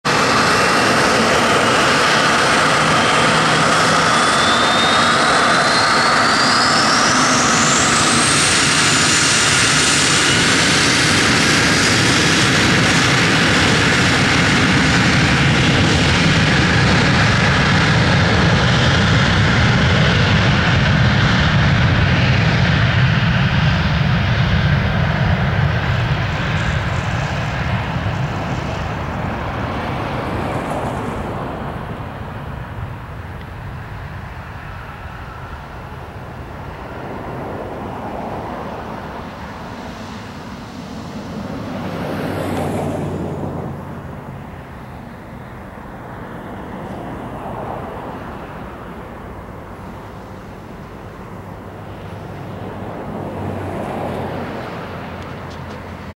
Southwest Airlines Illinois One N918WN 737-700 Takeoff Portland Airport (PDX)
Taking off of runway 28R.